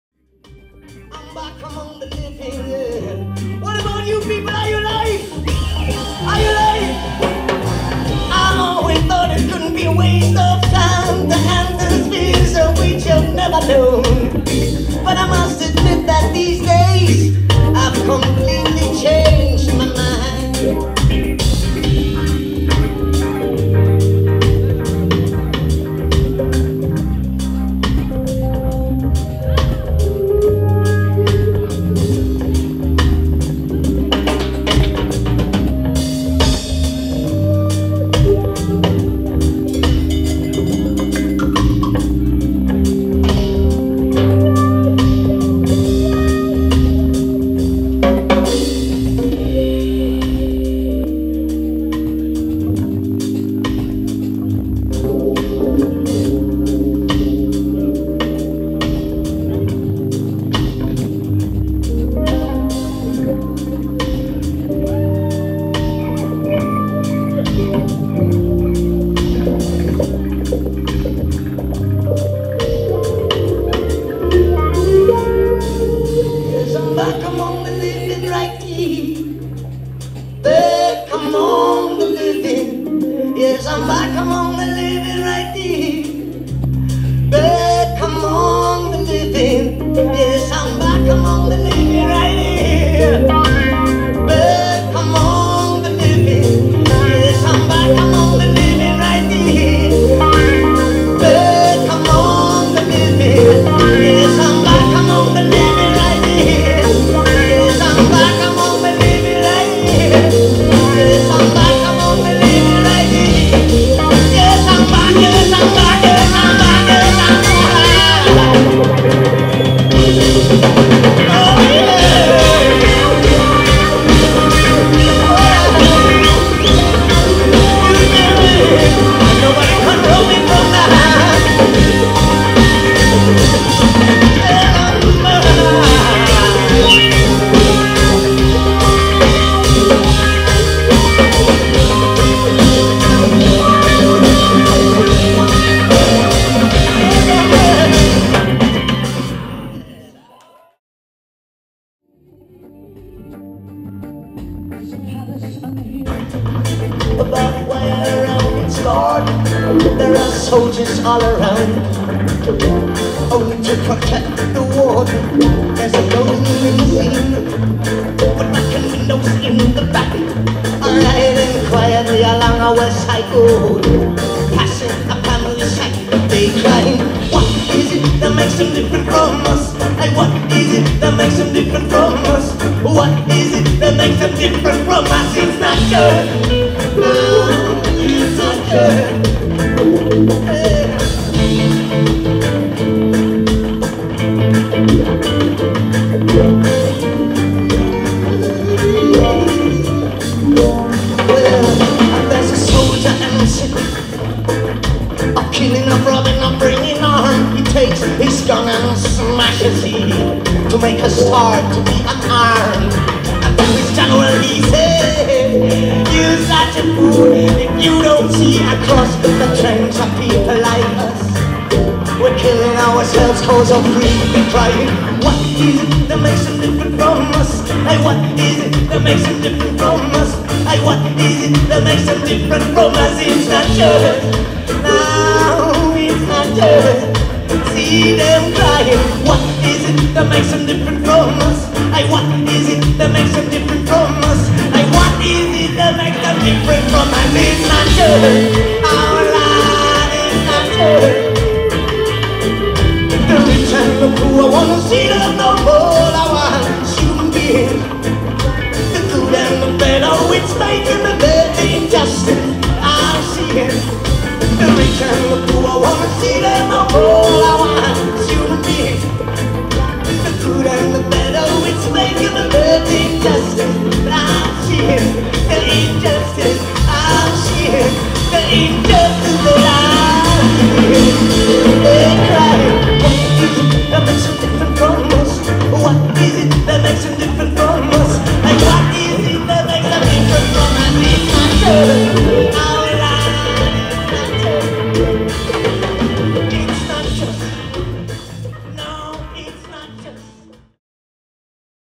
SEBASTIAN STURM @ BT59, BORDEAUX - ONE DROP FESTIVAL 10 MAI 2012
Podcast vidéo de Sebastian Sturm, au BT59 pour la première soirée de l'association Fokonsactiv' et du One Drop Festival. En attendant les montages multicams... Bravo à tout le monde pour cette super soirée, deuxième round le 31 avec les Jouby's.